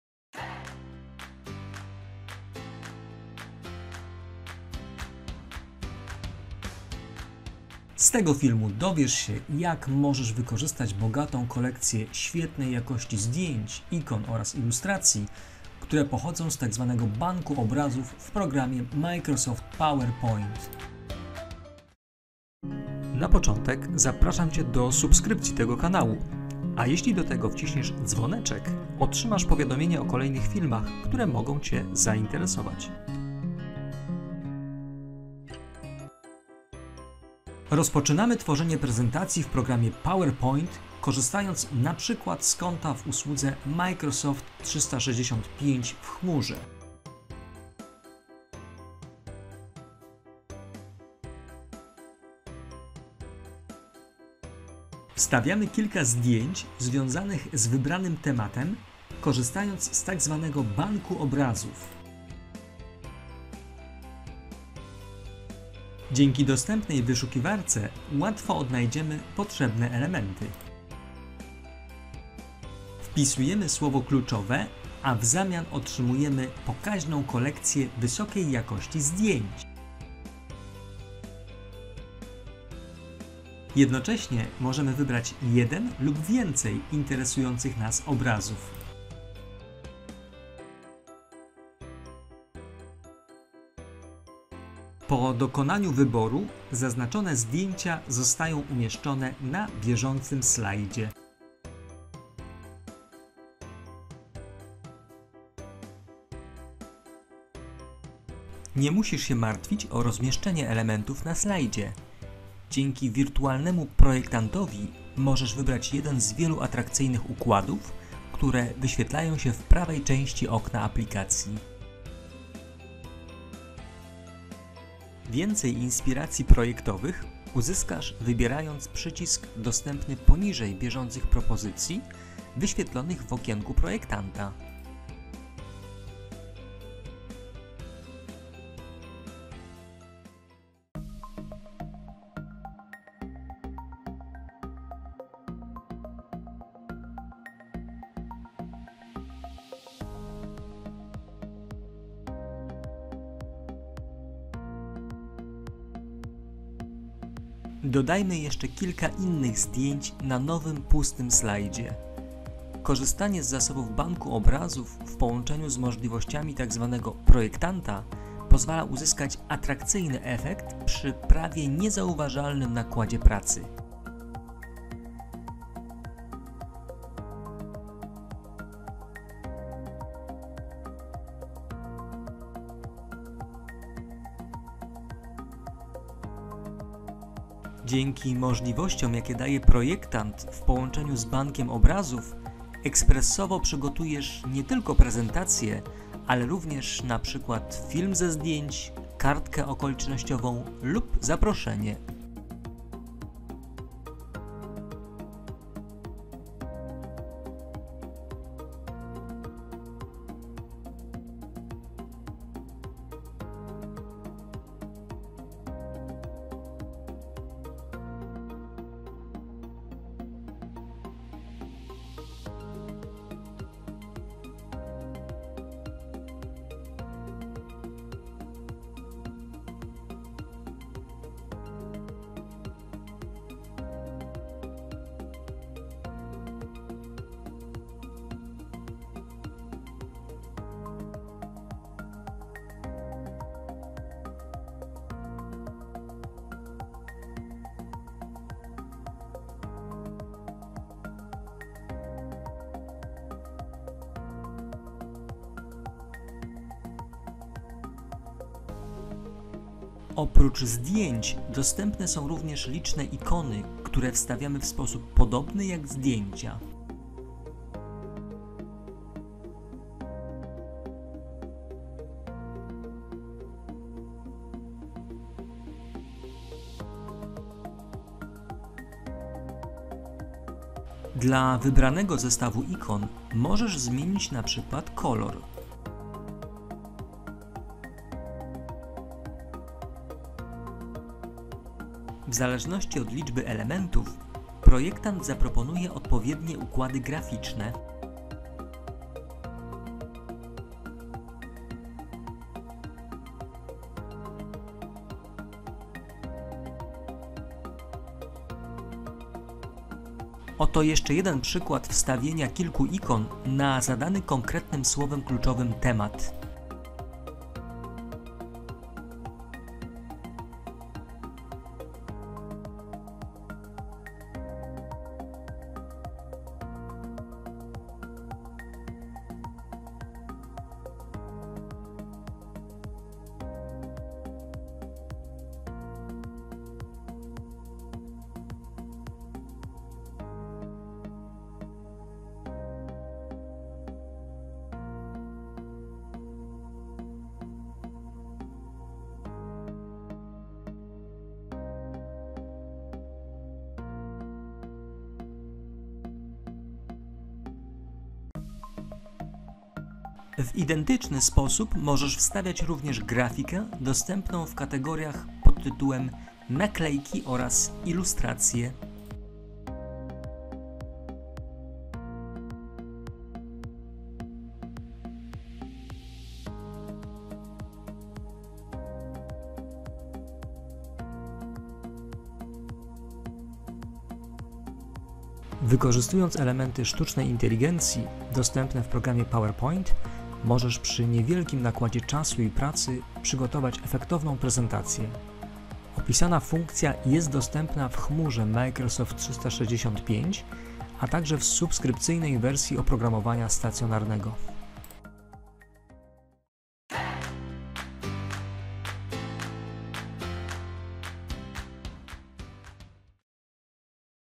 💝PowerPoint: Walentynki z Bankiem obrazów 💕
💡 W filmie zobaczysz, jak wykorzystać możliwości funkcji Projektanta oraz Banku obrazów w programie Microsoft Power Point (w wersji chmurowej lub stacjonarnej dla subskrybentów Microsoft 365) w celu uzyskania szybkiego i atrakcyjnego efektu w prezentacji.